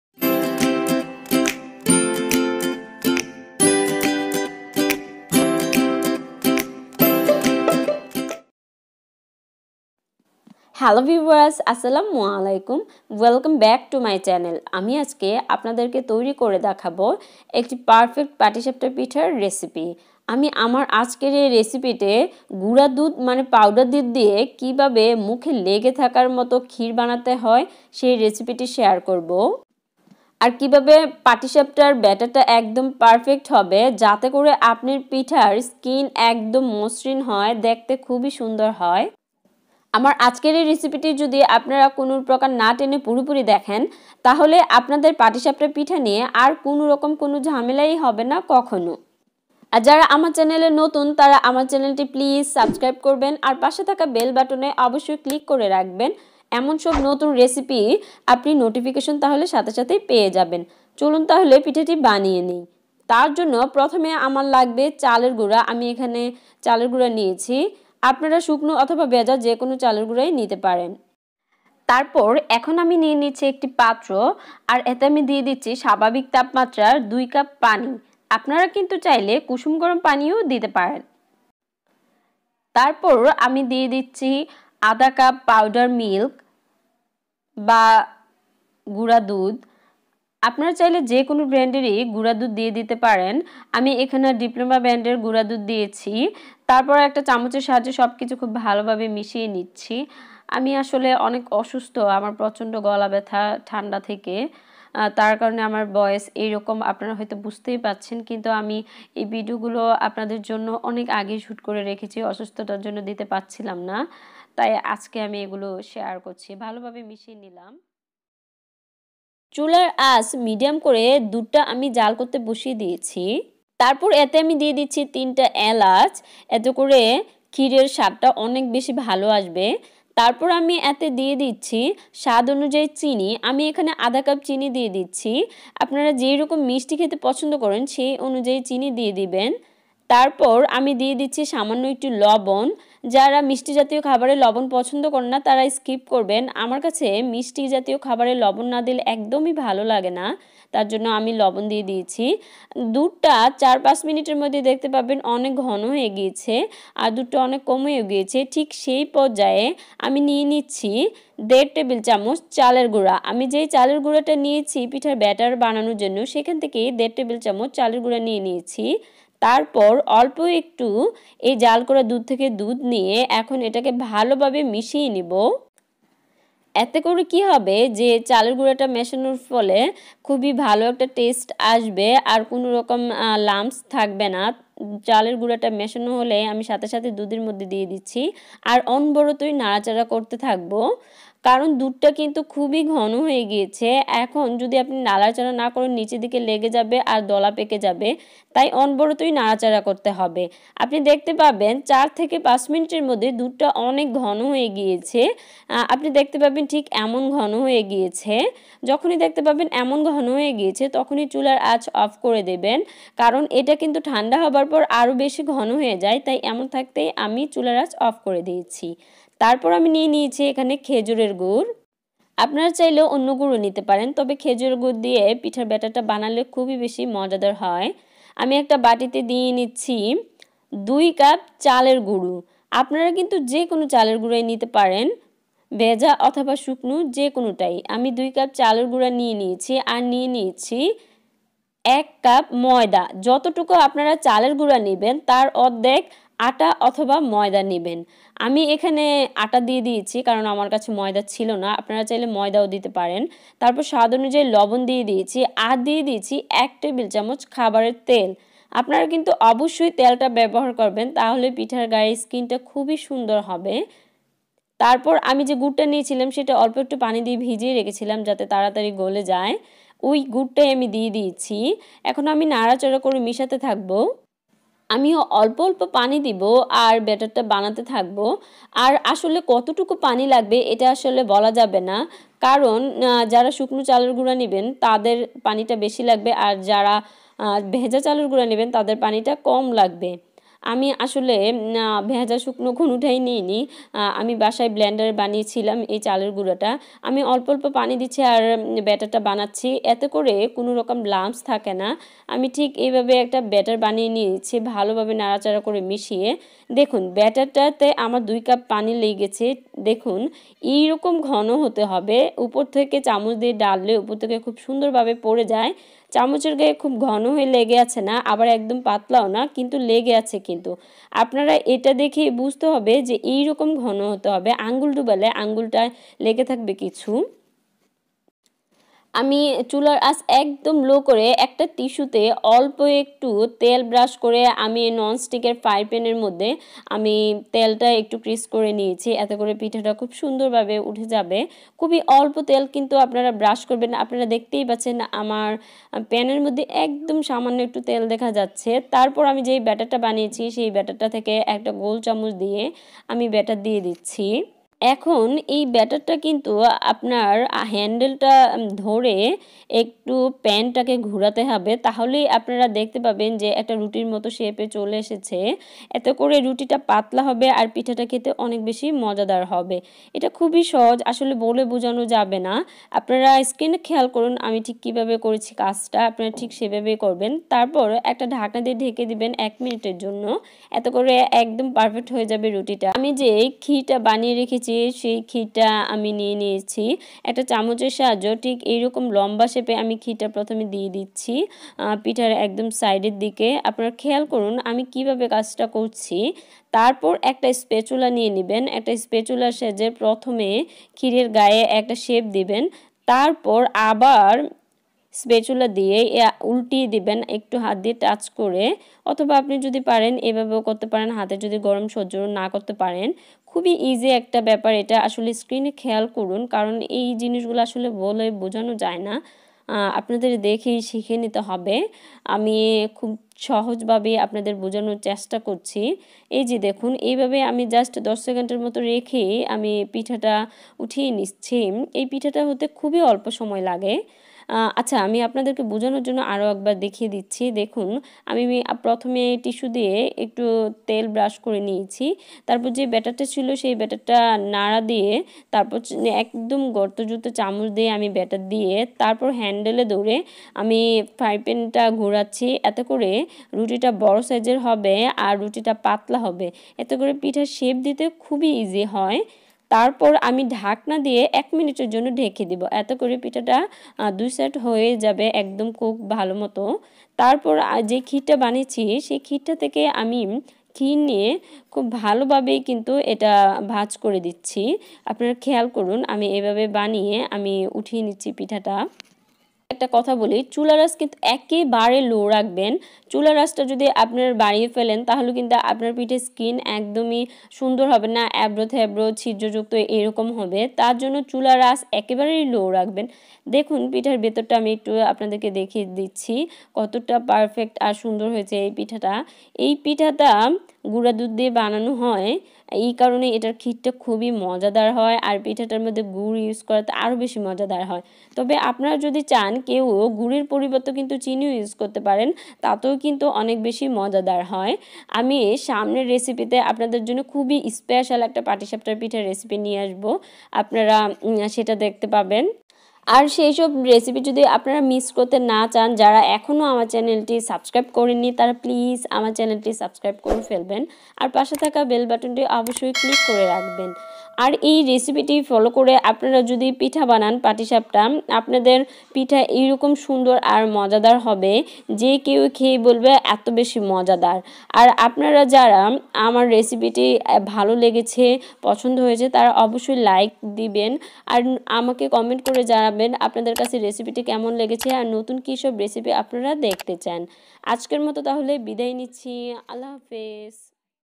২মিনিটে তৈরি পাটিসাপটা পিঠা,গুড়া দুধের ক্ষীরশায় ভরপুর মজাদার || সকল টিপস সহ Patisapta Pitha Recipe
উপকরণ ঃ
২কাপ পানি
২ কাপ আতব চালের গুড়া
১ কাপ আটা/ময়দা
১ টেবিল চামচ খাবার তেল
আধা কাপ গুড়া দুধ/ Powder milk
আধা কাপ চিনি
৩টা এলাচ
খেজুর গুড়
স্বাদমতো লবন